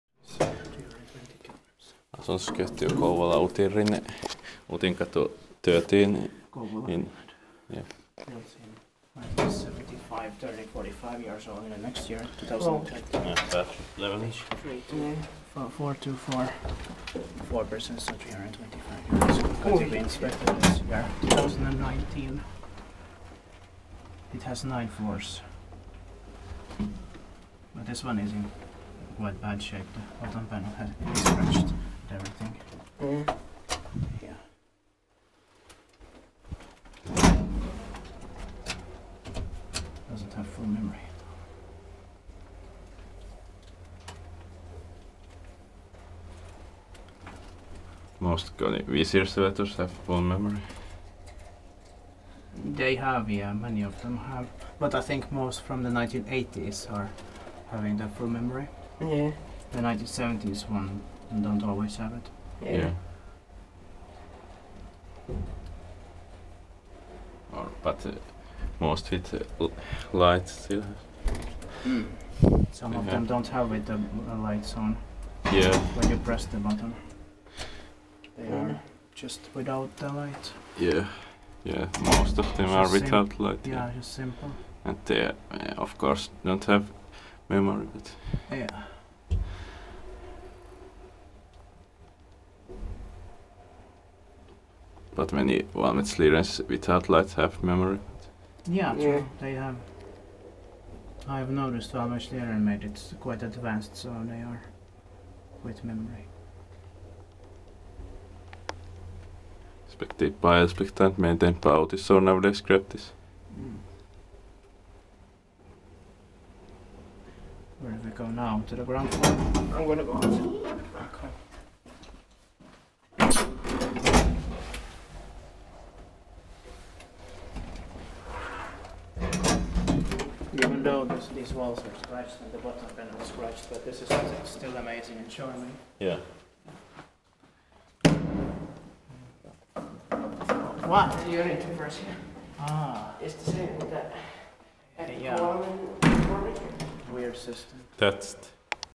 Old Kone traction elevator/lift in Utinkatu 13, Kouvola
Location: Utinkatu 13, Kouvola, Finland
Brand: Kone, Filmed with Elevators from Finland & Finnish Elevators 06
Year installed/modernized: 1975
Serial number: H 32424
Floors: 8(K, P, 1, 2, 3, 4, 5, 6, 7)
Capacity: 4 persons or 325 kg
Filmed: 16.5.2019